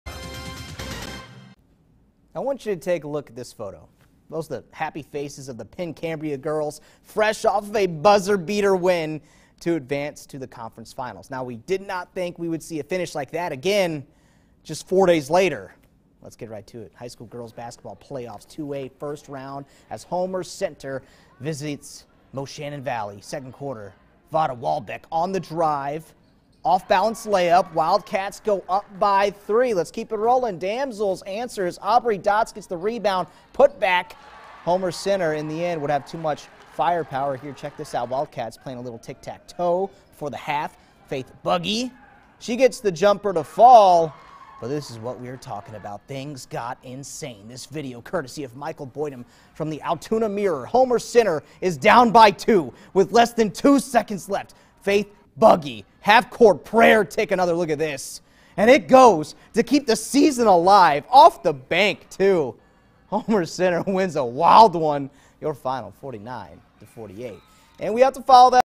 Homer-Center Takes Out Moshannon Valley on Buzzer Beater
See the highlights here